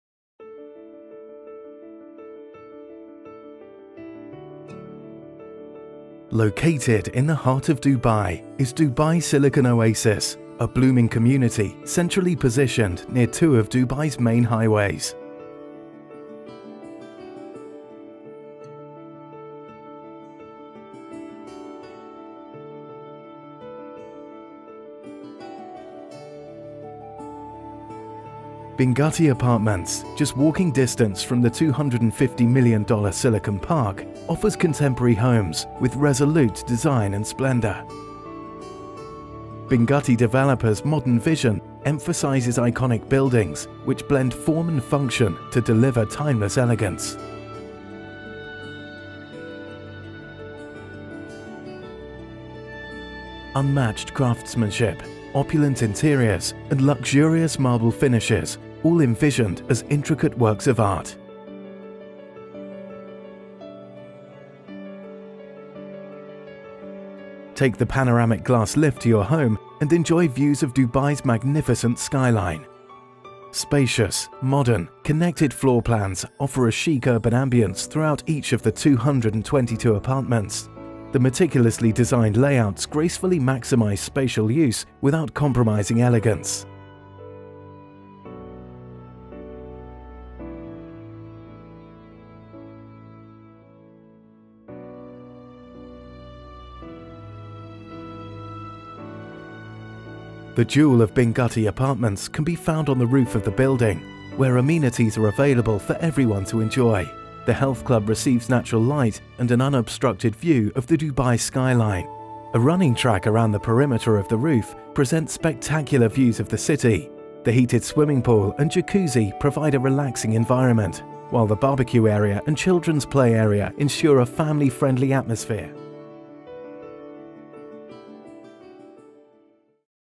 Binghatti Developers Launches Binghatti Apartments
• Binghatti Developers offers affordable properties with  groundbreaking architecture and design to be completed in less than one year.

• The brand offers affordable properties with world class materials and finishing starting from AED 425,000.

• The 222 apartments will be centrally located just 10 minutes from Downtown Dubai in Dubai Silicon Oasis.